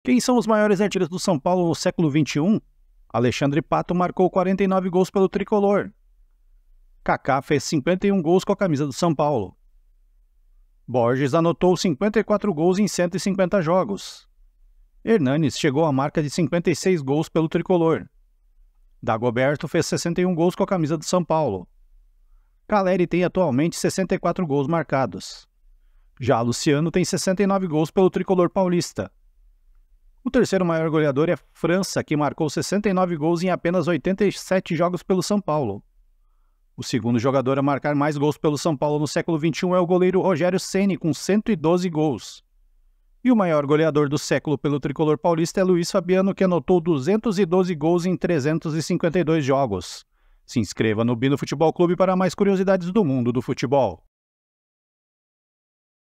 MAIORES ARTILHEIROS DO SÃO PAULO NO SÉCULO 21 (2001-2024)
Você sabe quem são os maiores artilheiros do São Paulo no século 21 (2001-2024)?

212 – Luis Fabiano (352 jogos)
112 – Rogério Ceni (906 jogos)
69 – França (87 jogos)
69 – Luciano (209 jogos) *
64 – Calleri (165 jogos) *
61 – Dagoberto (241 jogos)
56 – Hernanes (330 jogos)
54 – Borges (150 jogos)
51 – Kaká (155 jogos)
49 – Alexandre Pato (142 jogos)
* Em atividade pelo clube